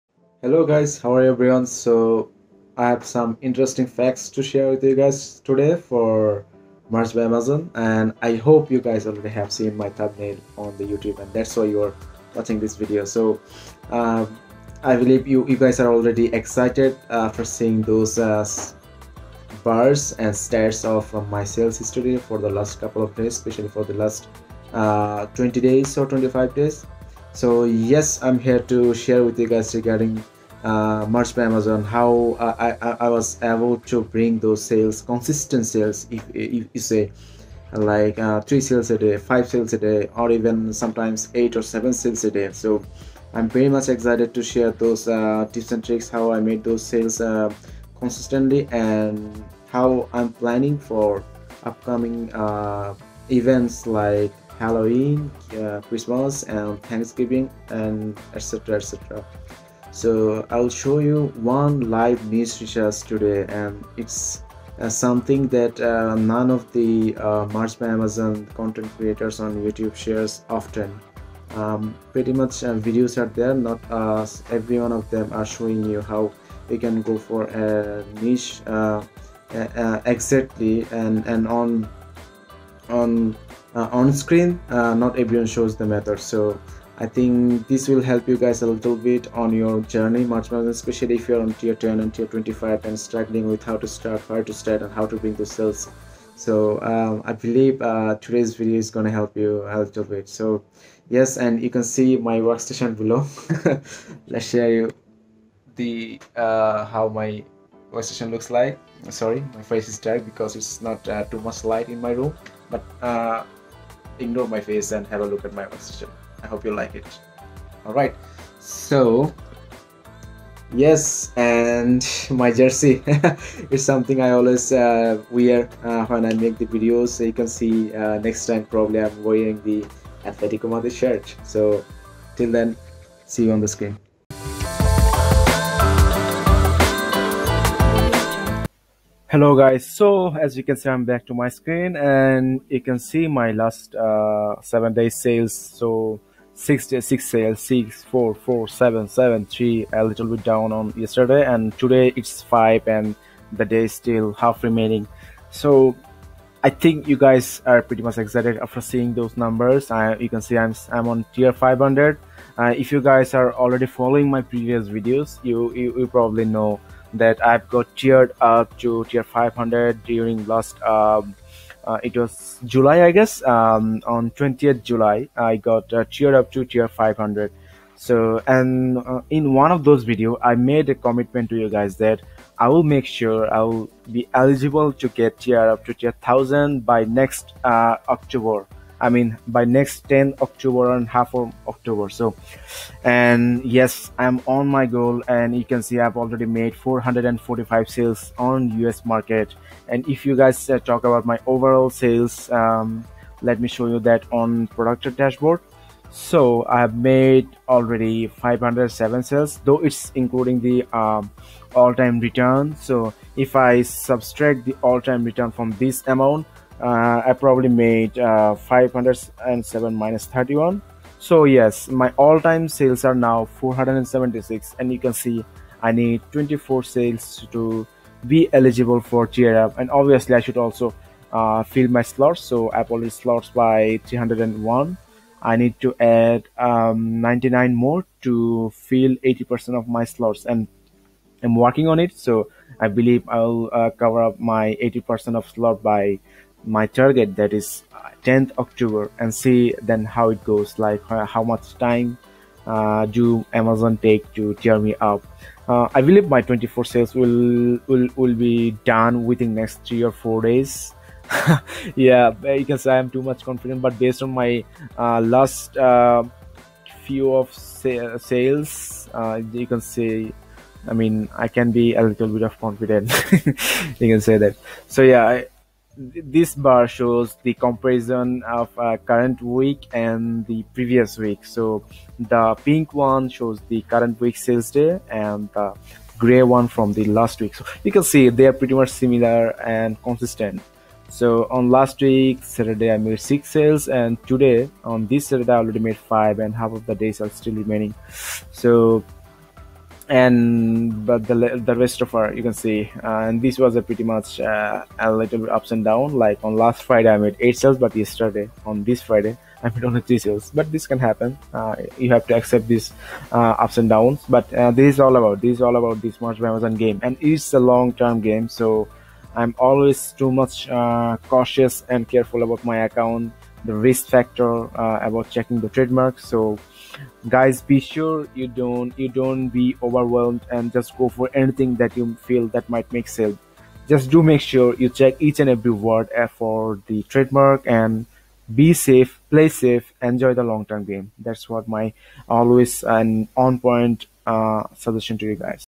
Merch by Amazon 2022 - Live Niche Research Step by Step (PART-01)
In this series of Merch by Amazon videos, I will show you guys how to research sellable low competition niches, step by step. I divided the videos into Episodes and hope you will enjoy them and learn something from me.